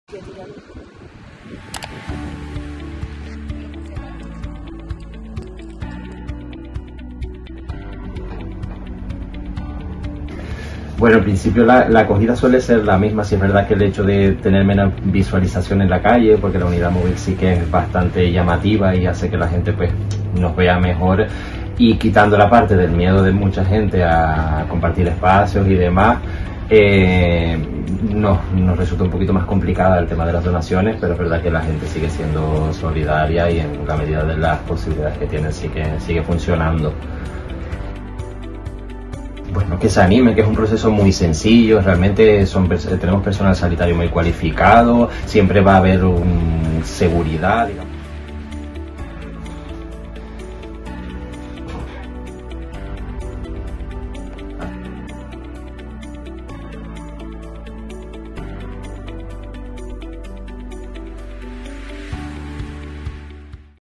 Nuevos puntos de extracción de la Campaña de Donación de Sangre en la ULL // Periodismo ULL
El Instituto Canario de Hemodonación y Hemoterapia da comienzo a otra campaña de donación de sangre en la Universidad de La Laguna. Esta cuenta con unidades móviles, que se trasladaran cada semana, y salas ofrecidas por los campus universitarios.